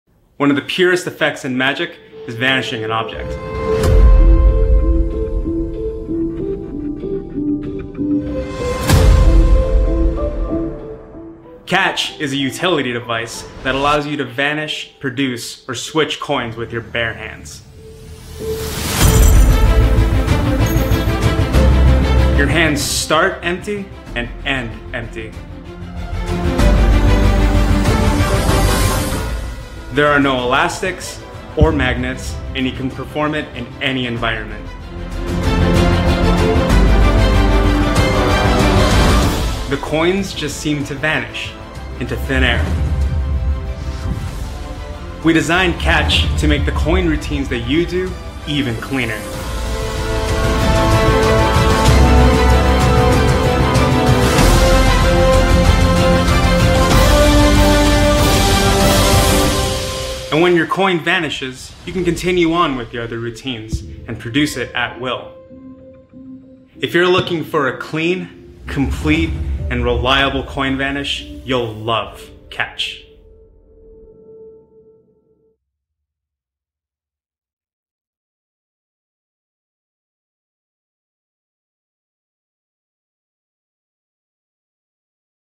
Catch by Vanishing Inc Trick
"Catch" gives you magical powers that once felt like they were only possible on the big screen.

"I love this gimmick. It does all of the hard work while you get to take all of the credit."
- Tom Elderfield

Close your eyes and picture yourself being able to truly make a coin vanish from your hand. Really take a moment to envision what it would look like if you had magical powers.

Got it?

Now, open your eyes and watch the trailer. For many of you, what appears in that video is probably quite similar to what you created with your mind movie. And that's exactly what Vanishing Inc. was going for when they created "Catch".

This revolutionary gimmick allows you to place a coin on your legitimately empty palm and cleanly close your fingers around it before opening them just a brief moment later to show that the coin has completely vanished. No fishy movements or awkward tells. Just pure magic.

"Catch" can be used with either hand and works with all types of pocket change from a variety of different countries and currencies. You can even borrow or sign the coins. The gimmick is easy to use and doesn't have any fragile moving parts or magnets.

Once the coin is secure in the "Catch" gimmick, an innovative locking system keeps it secured in place until you wish to release it. That's right. "Catch" not only vanishes coins, but allows you to produce them as well-making it the perfect utility for transpositions, changes, or any of your other favorite coin tricks.

The coolest part of this gimmick is that you can do it with your sleeves rolled up (but you will need to wear something like a watch or wrist strap to hold the gimmick). It's an unobtrusive gimmick that is ready to go as soon as you need it.

Each "Catch" gimmick comes with access to a nearly 40-minute download offering a variety of handlings and uses.